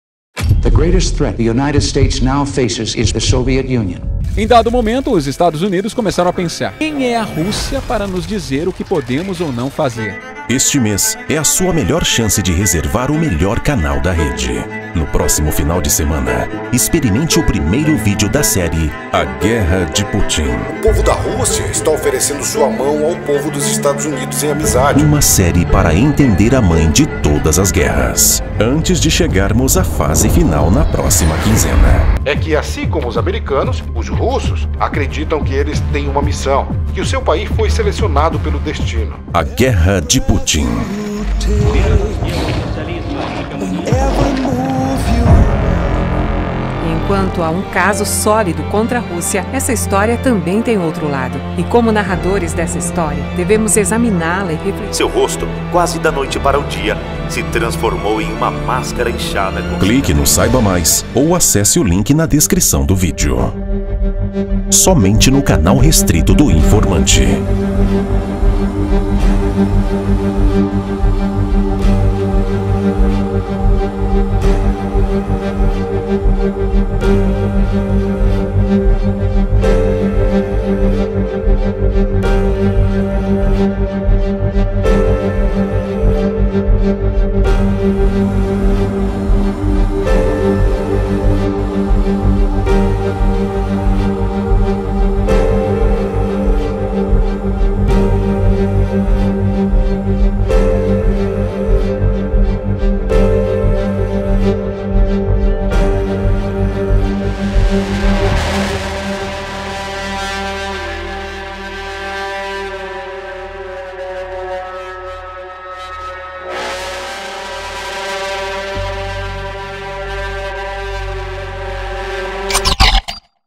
A guerra de Putin ( O INFORMANTE ) Trailer
Prepare-se para uma aula de geopolítica. No vídeo do canal restrito do informante tudo o que você precisa saber sobre a guerra na Ucrânia. As opiniões precisas, as melhores fontes que não aparecem no Brasil e análises de quem sabe ler a Rússia em todas as suas entrelinhas. As tramas, o jogo do poder, onde estamos e para aonde vamos no que se tornará a mãe de todas as guerras. Prepare-se pois nessa guerra todos serão afetados. Mas estar informado te colocará um passo à frente na fase mais importante que se inicia: a fase da sobrevivência.